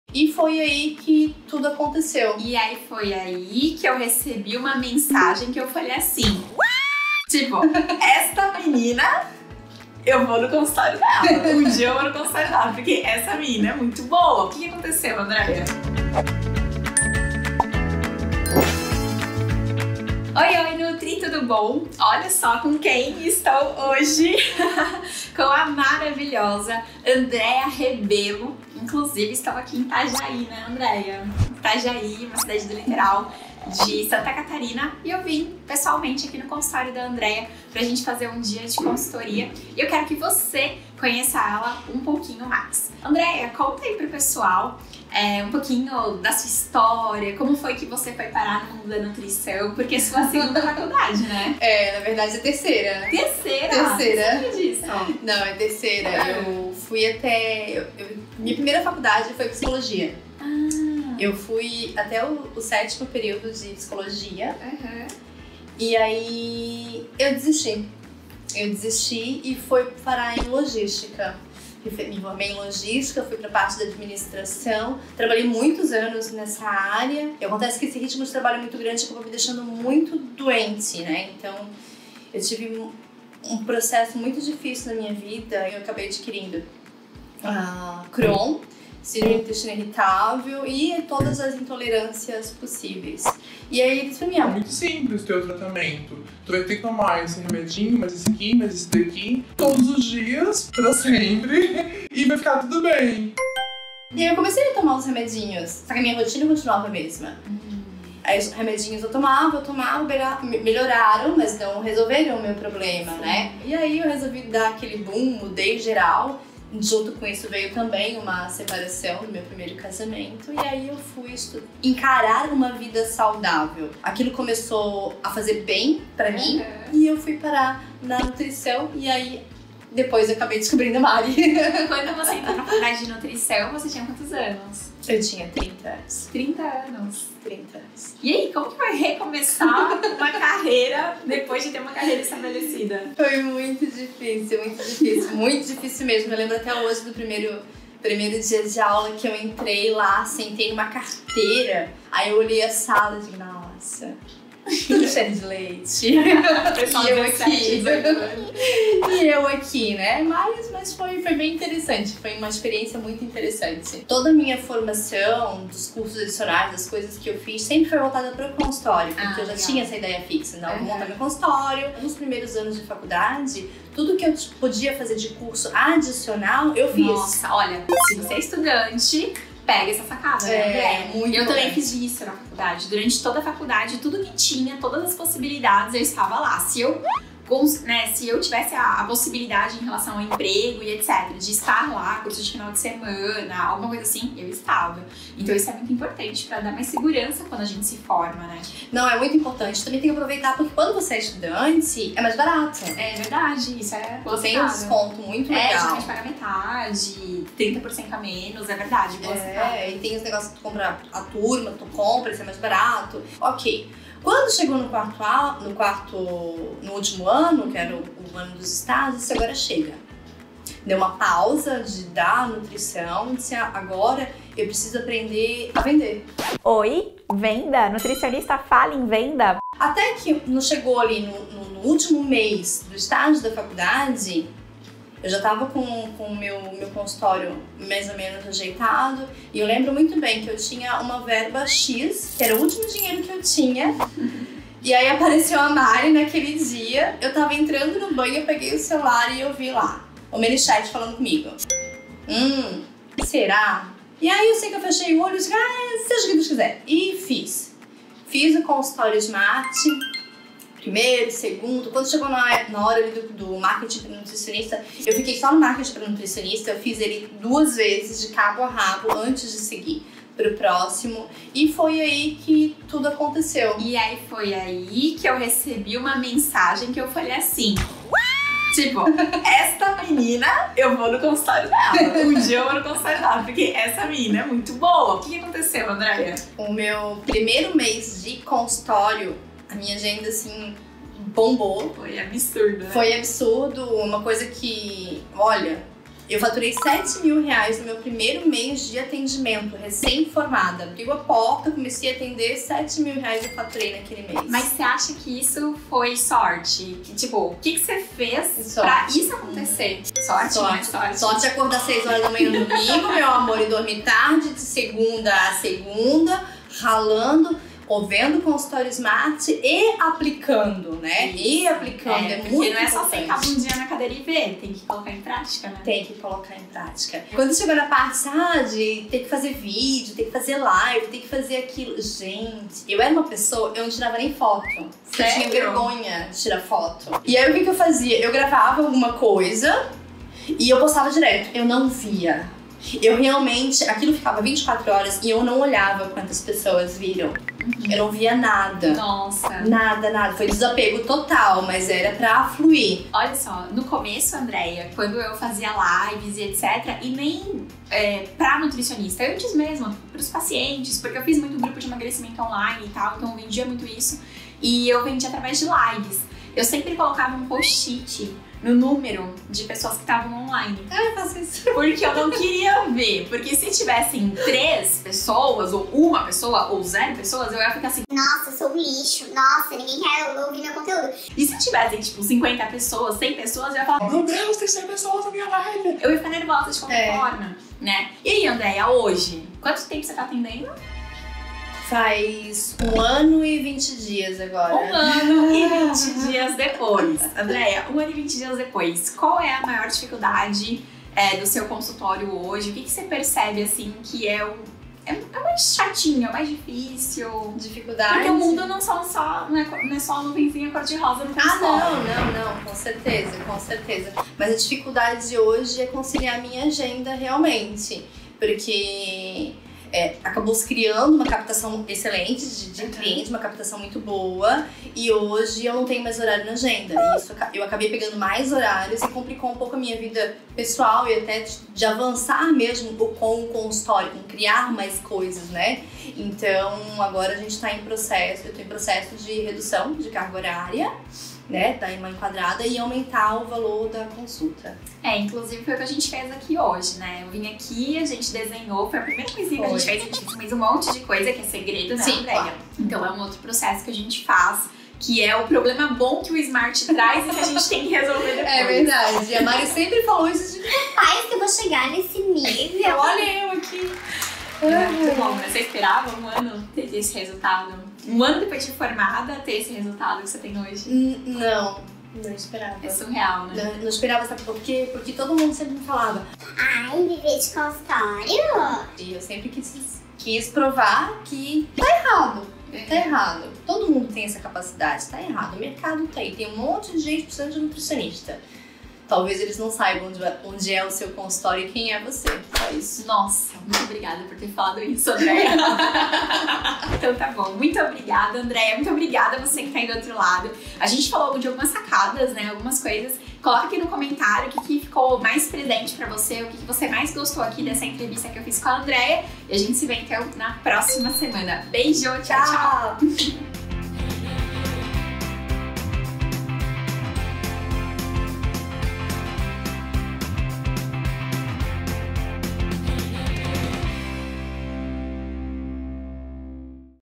NUTRI recém-formada com consultório BOMBANDO? Com Andrea Rebello | Mari Fazzi | Amor em Nutrir
Nutri, depois da Andrea fazer uma enorme transição de carreira, ela ainda ouviu MUITA gente falando que VIVER DE CONSULTÓRIO era impossível.
Mas ela resolveu não dar ouvidos a isso e provar que esse pensamento estava totalmente errado.
Bom, assiste aí então pra você entender melhor a volta por cima que a Andrea deu!

Gostou do vídeo? Então Curta e compartilhe!